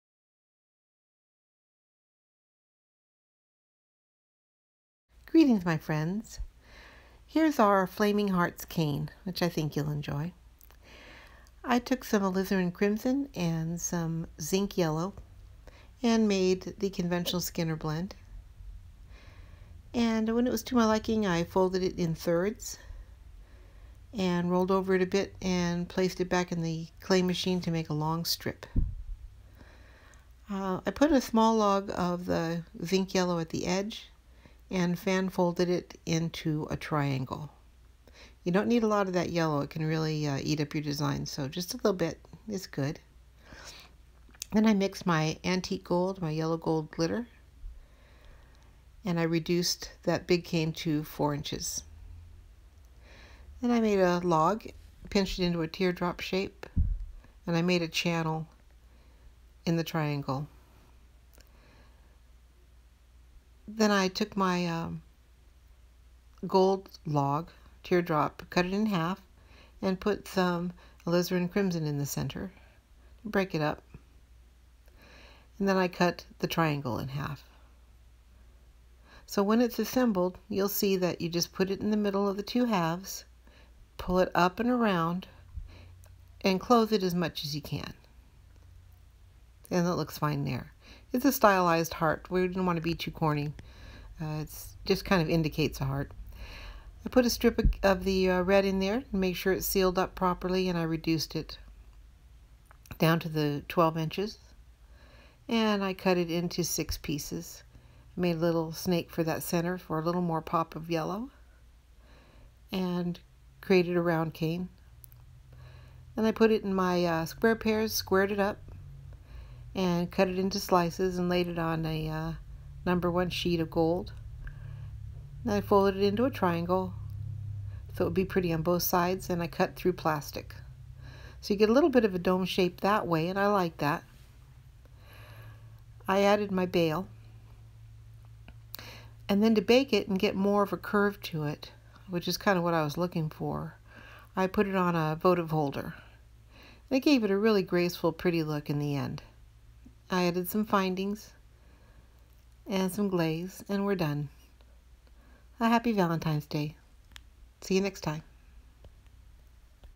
Polymer Clay Flaming Hearts Cane
Use your Flaming Hearts Cane to make jewelry, journals or home decor. I used Sculpey Premo Alizarin Crimson, Zinc Yellow, Antique Gold and Yellow Gold Glitter.